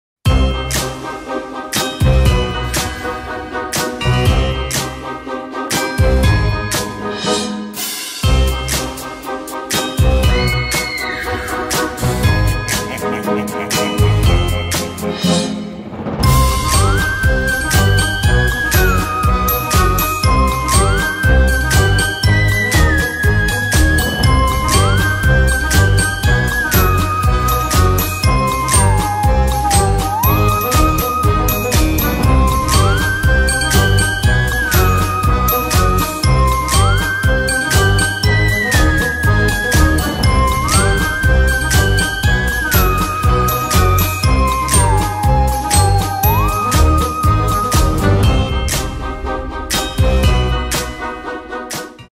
Halloween Party "Dia de los Muertos" - Eldoradio x Orange
Vos cauchemars sont devenus réalités le 31 octobre dernier avec notre Halloween Mystery Box. 📦😈 Avez-vous osé mettre votre main à l'intérieur ? 😱 On découvre cela ensemble !

Merci Eldoradio pour cette chouette soirée ! Nous étions heureux d'en être partenaires.